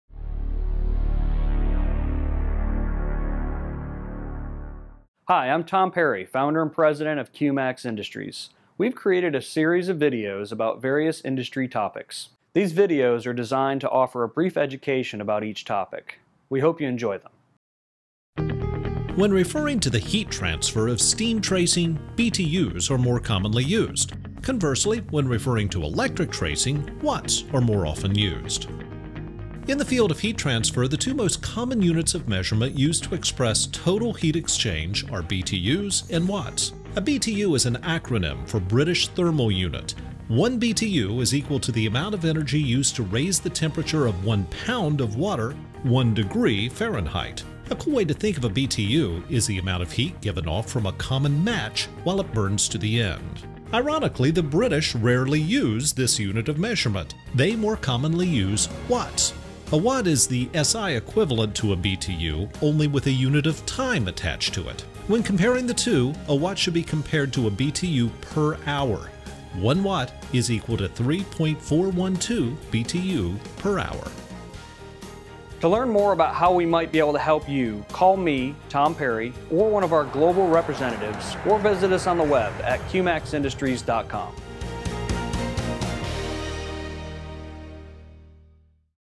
Heat Transfer Series Video 2: BTUs vs Watts
In this video we discuss the difference of BTUs and Watts and how they relate to heat transfer.